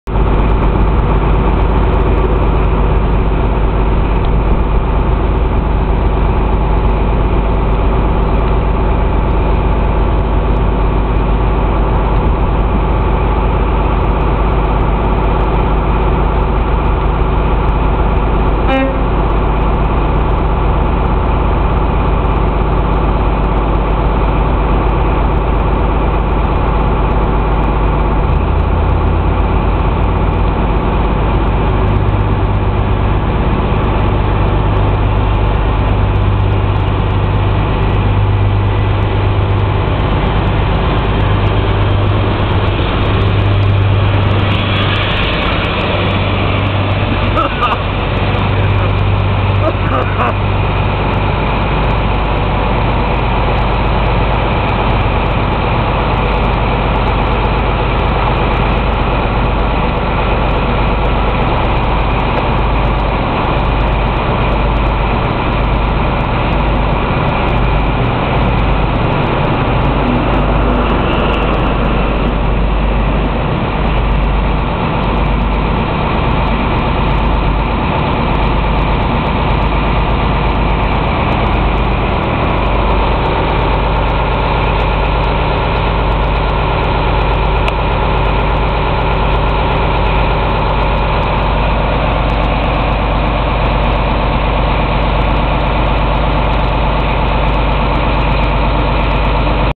{HQ} **100th Public Video** EMTs HST 43064 uber-clag during departure from St. Pancras [11/01/2009]
East Midlands Trains HST 43064 leaving Platform 3 at St. Pancras Domestic decides to show off just a little bit by clagging away.

Having walked up the platform to see this HST departing, got to the front power car and it was clagging away already and that's only on idle.

The driver gave a little tone (didn't even ask for it, but thanks driver!) and then off the HST goes clagging away like crazy. And yes at about 0:50 that was me coughing as it clagged even more past me and made me cough. Also watch to the end as it even clags more once clear of the platform!

This video was filmed on 11th January 2008.